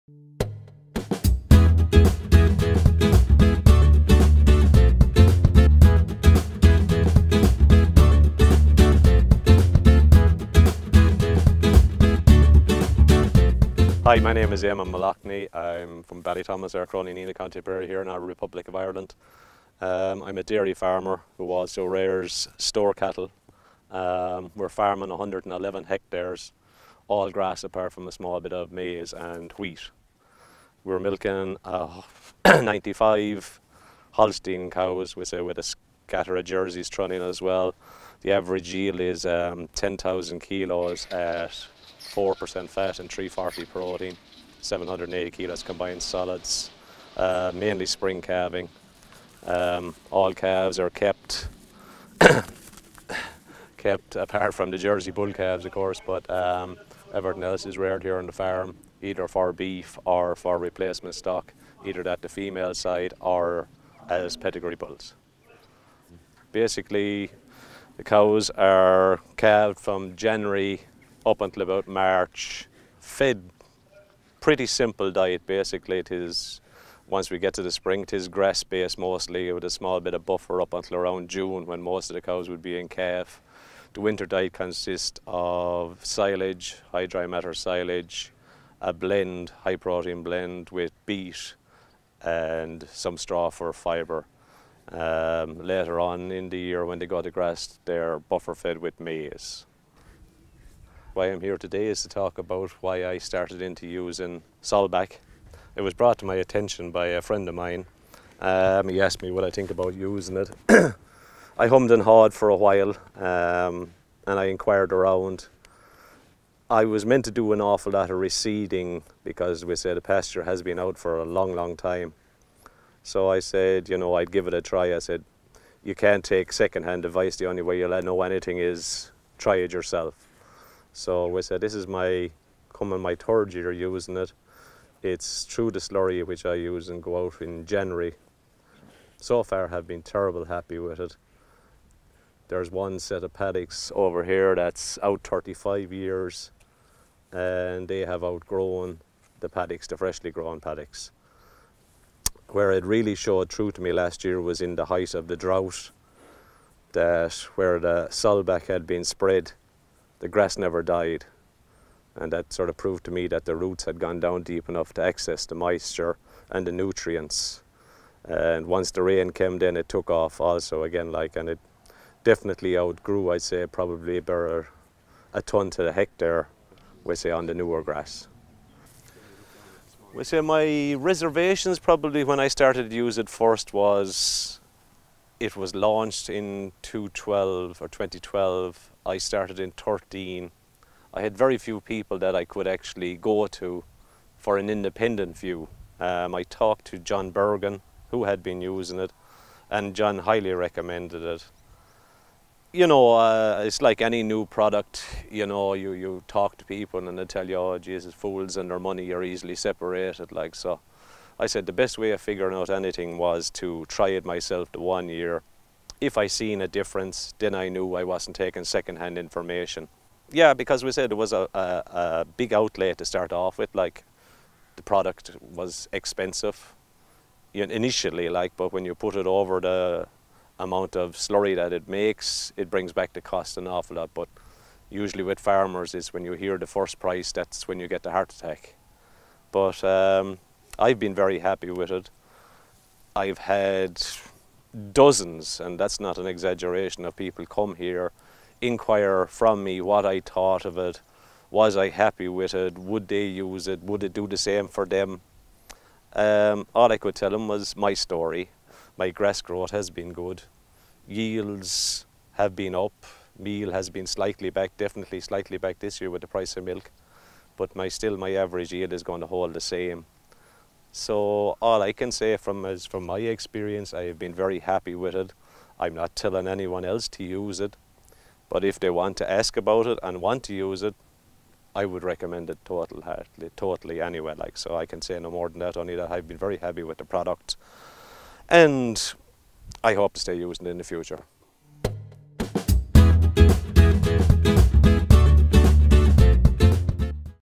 Eamon McLoughney. Dairy farmer from Ireland - SOBAC
Eamon McLoughney. Dairy farmer and Holstein breeder in Country Tipperary tells us about his experience with BACTERIOSOL and BACTERIOLIT.